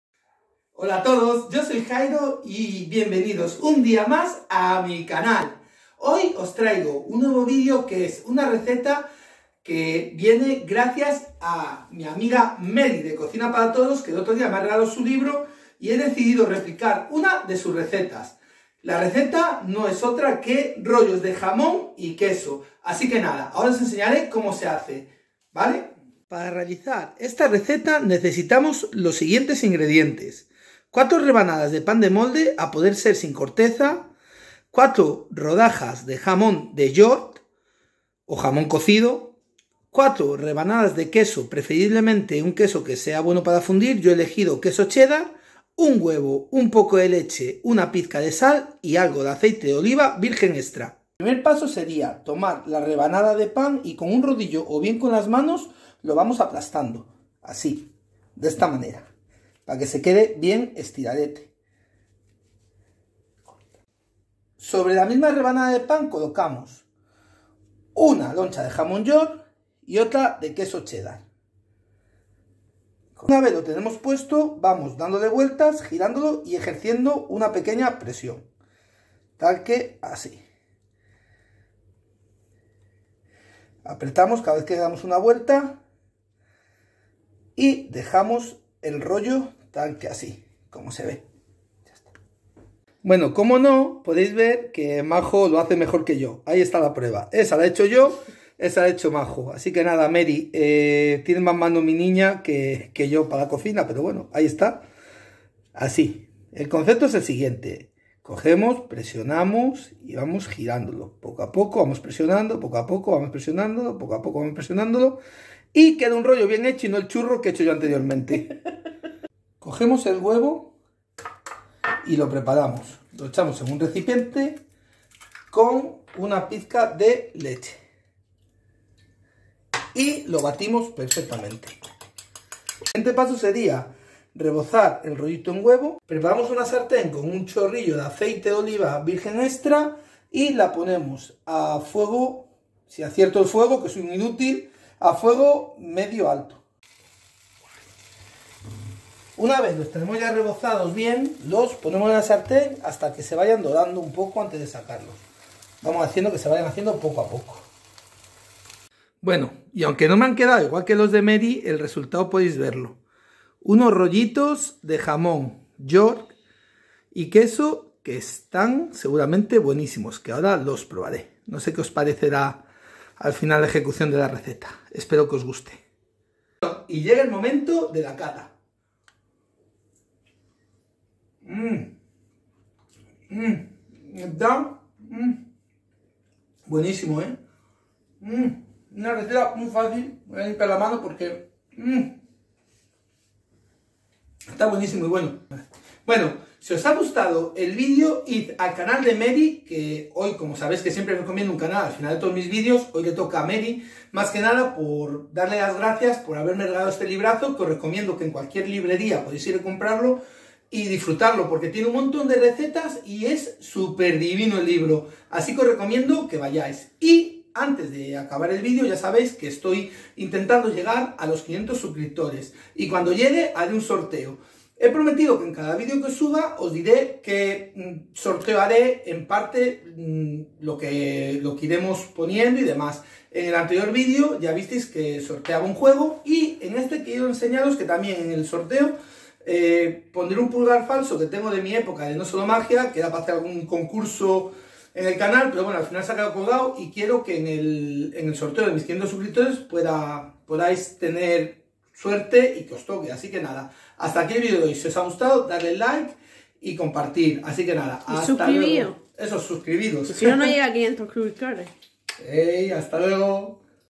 ROLLITOS de YORK y QUESO: Replicando una receta del libro de MERY de COCINA PARA TODOS
Bueno el video de hoy es un video muy especial pues Mery del canal Cocina para todos aqui os dejo el link de su canal.
Me ha regalado su libro y no he podido resistirme a hacerle un homenaje replicando una de sus recetas.
Los ROLLITOS DE JAMON YORK Y QUESO.
Ingredientes:
4 rodajas de pan de molde sin corteza
4 lonchas de jamón york o jamón cocido
4 lonchas de queso, preferiblemente Cheddar
un huevo, un poco de leche, sal y aceite de oliva extra...
Paso a paso tenéis la receta en el video y también os invito a que conozcáis el canal de Mery que es una pasada, así como enseñaros el nuevo regalo para el sorteo ni más ni menos que un pulgar falso para hacer magia, espero que os guste el video.